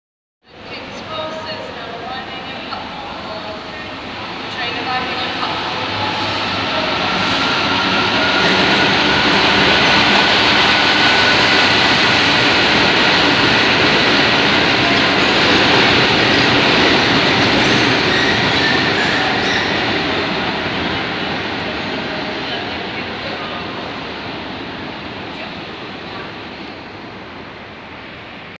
Freightliner Convoy passing Darlington 1/7/17
Freightliner's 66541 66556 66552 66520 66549 form the 088F Leeds Balm Road - Millerhill engine move, seen here passing through Darlington on there journey northwards on 1/7/17